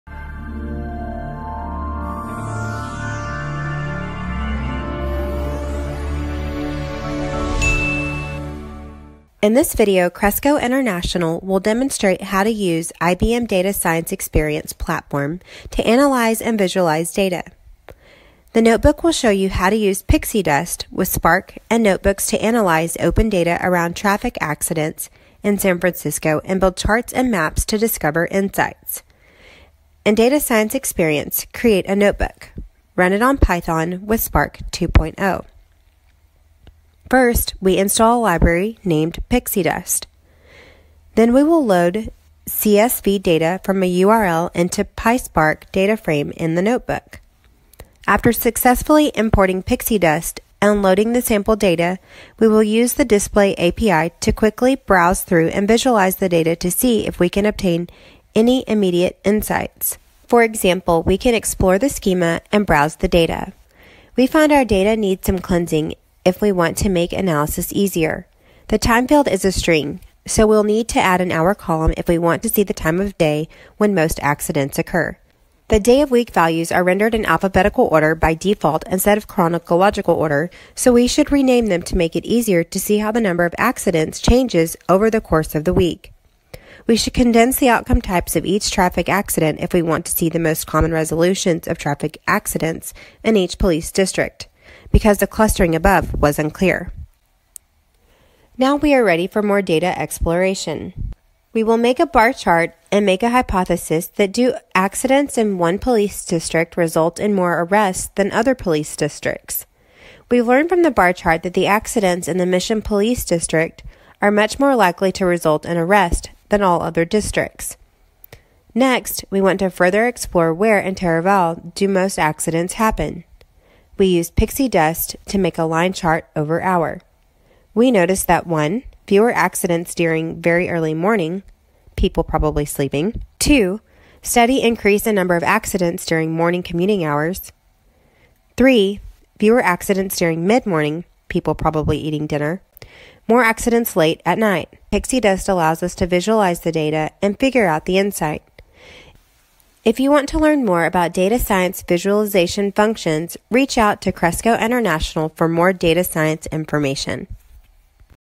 Analyze Traffic data to find data correlations using IBM Data Science Experience (DSX)
This notebook shows how to use PixieDust with Spark to analyze data around traffic accidents in San Francisco, finding correlations in police arrests and building vizualizations using PixieDust in IBM Data Science Experience (DSX).